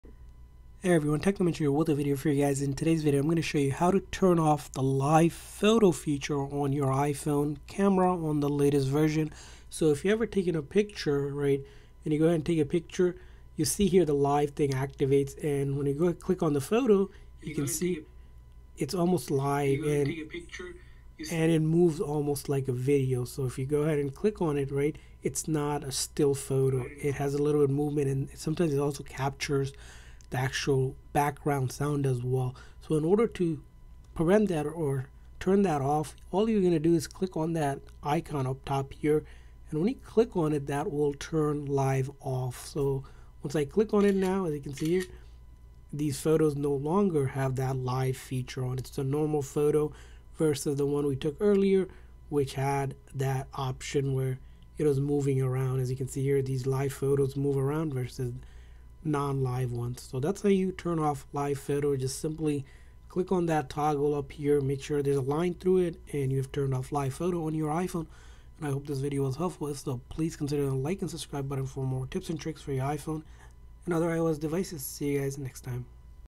How to Turn Off Live Photo on iPhone
In this iPhone tutorial we show you how to turn off live photo on your iPhone camera.